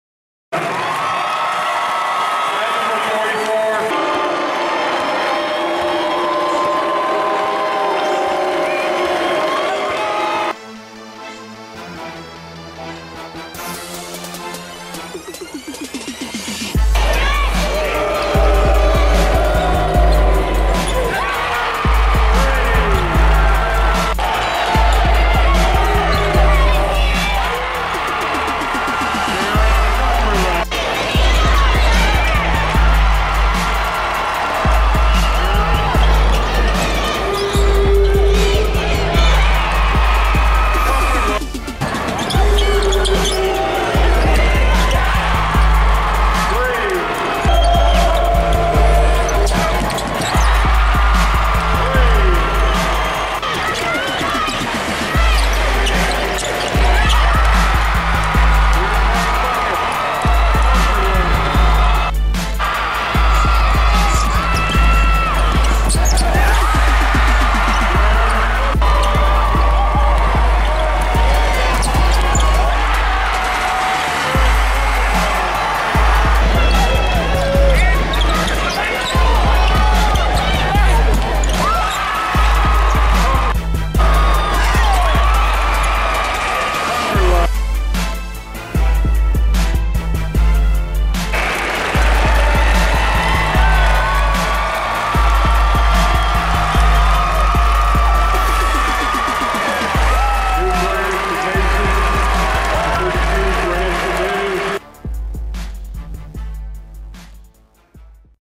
Jarron Cumberland Drops 35 in District Final Win
Jarron Cumberland scores 26 second half points to lead Wilmington to a 63-38 win over Mason in the Division 1 District Championship at Wright State. Cumberland also had 5 rebounds and 3 assists.